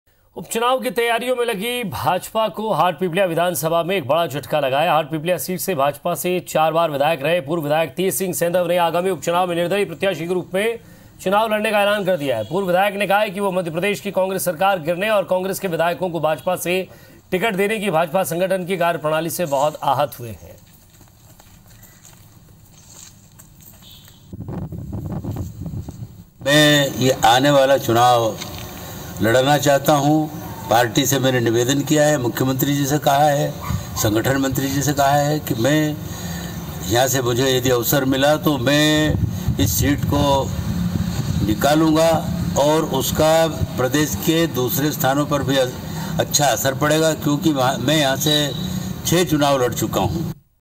BJP को लगा एक और झटका | Hatpipliya से 4 बार MLA रहे Tejsingh Sendhav लड़ेंगे निर्दलीय चुनाव
IBC24, BJP को लगा एक और झटका, Hatpipliya से 4 बार MLA रहे Tejsingh Sendhav लड़ेंगे निर्दलीय चुनाव